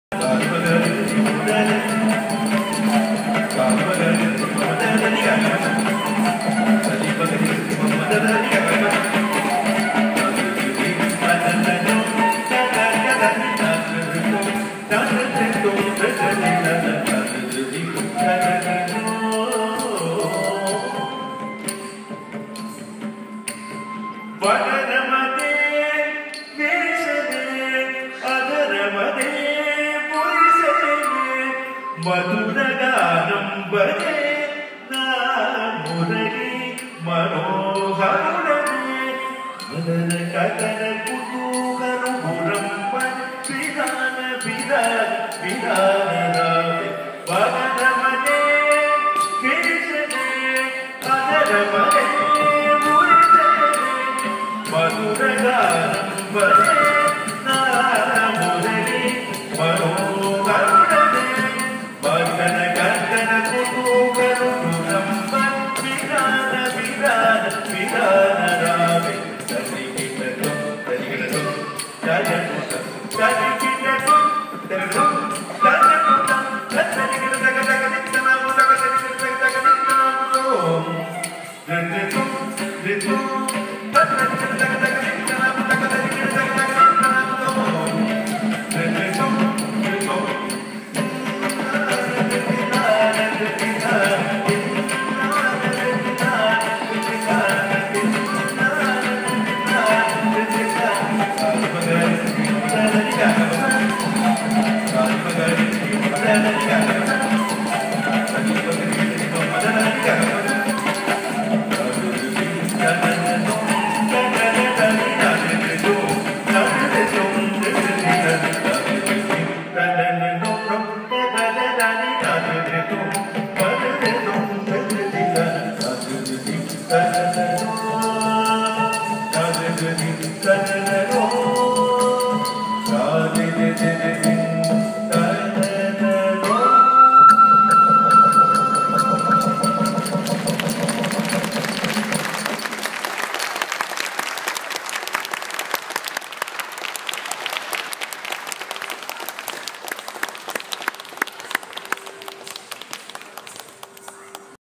"Bharatanatyam Arangetram" performed by Samira Rayna Ali
Broken Arrow PAC , Oklahoma , USA